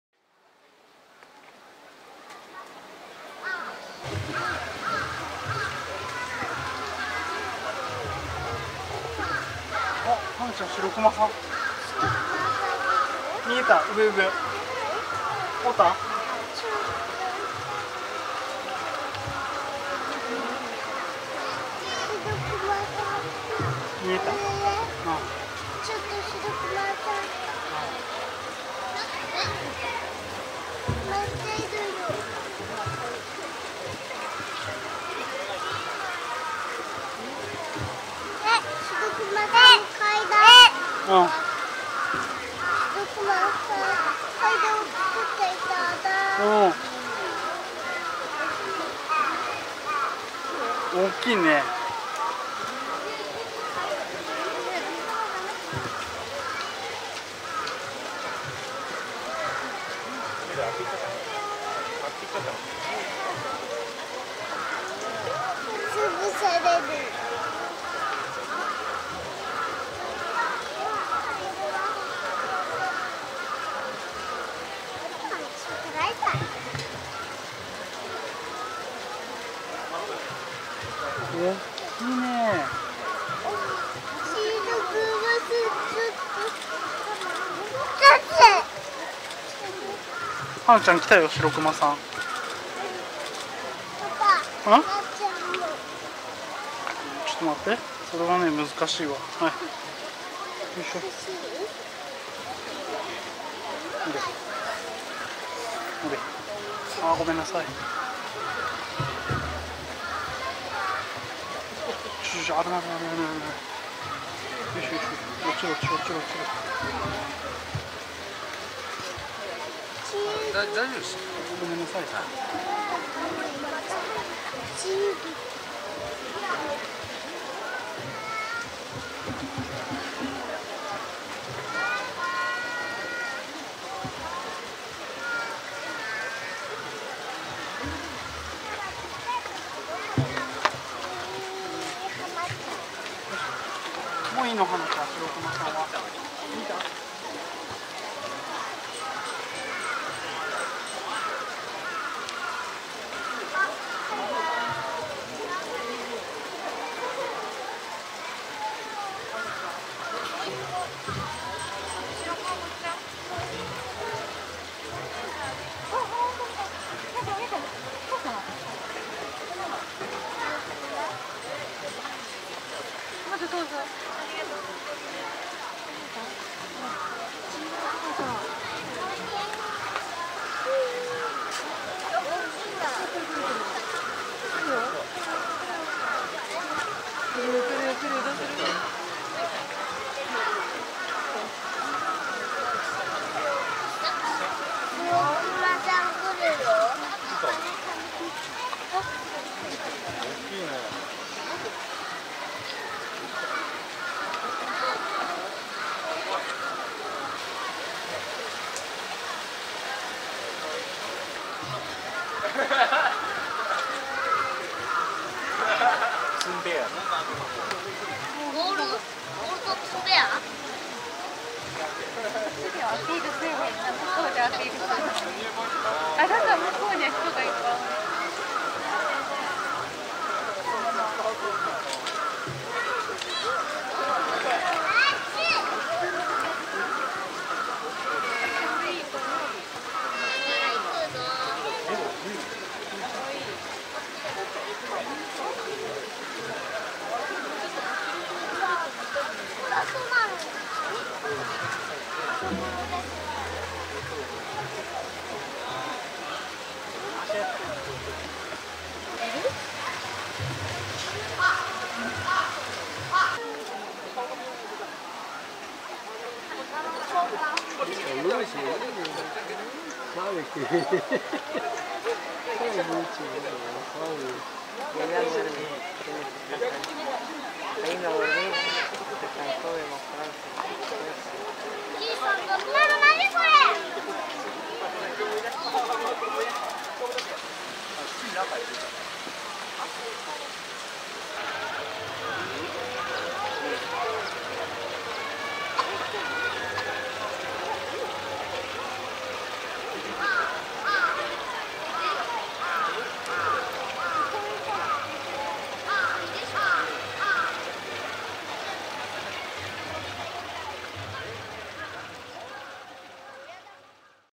その後のフブキ 〜 2023/06/04 日曜日【東山動物園】
2023年6月4日 日曜日 名古屋市にある東山動植物園のホッキョクグマのフブキの様子を収録した動画です。

午後からのおやつタイムの後､フブキが外に手で来た時に撮影した映像を編集しました。

Sunday, June 4, 2023 This is a video of the polar bear Fubuki at the Higashiyama Zoo and Botanical Gardens in Nagoya.

After snack time in the afternoon, I edited the video taken when Fubuki came outside with his hand.